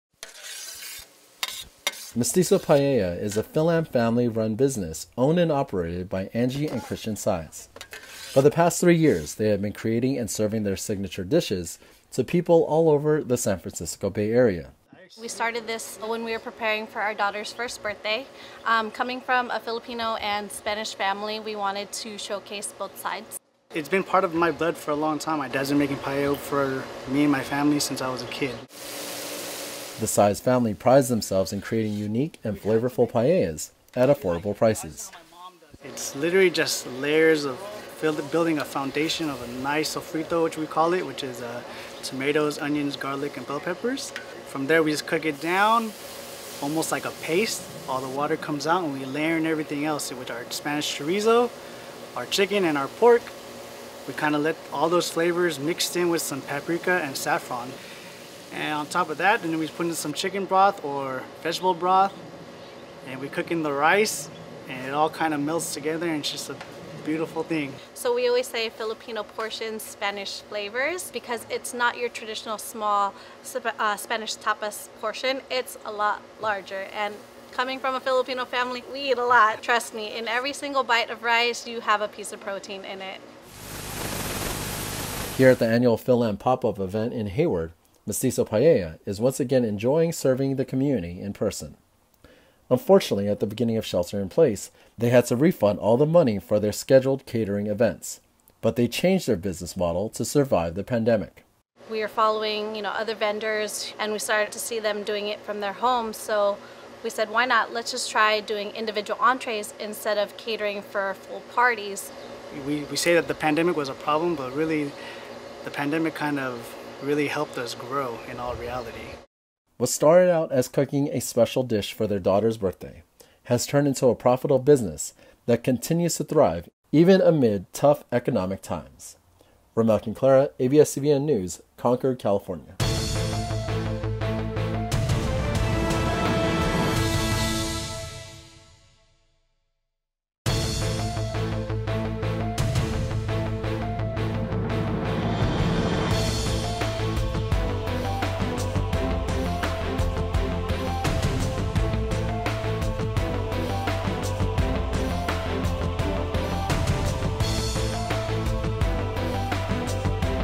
Fil-Am couple shares how their Paella business flourished amid pandemic | TFC News California, USA
For the past three years, a Fil-Am family has been serving flavorful Paellas to people in the San Francisco Bay Area. They share how their business was able to roll with pandemic-induced changes and managed to thrive despite tough economic times.